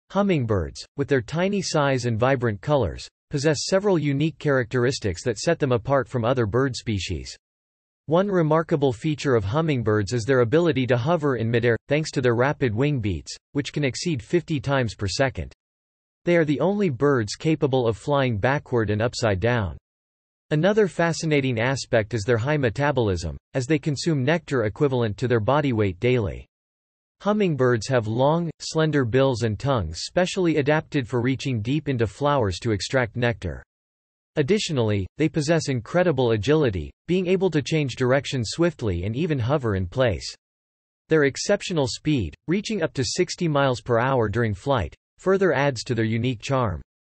Hummingbird - Masters of Flight and Elegance
Embark on a captivating journey into the world of hummingbirds, the smallest and most mesmerizing birds on the planet. Discover their extraordinary abilities, from their incredible hovering skills to their lightning-fast flight. Explore the fascinating adaptations that enable them to reach nectar deep within flowers and sustain their high-energy lifestyle. Learn about their vibrant plumage and the significance of their iridescent feathers. Witness their unique courtship rituals and the astonishing displays of aggression in defending their territories. Join us as we delve into the secrets of these tiny avian wonders, uncovering the marvels of their behavior, ecology, and conservation. Get ready to be enchanted by the elegance and beauty of hummingbirds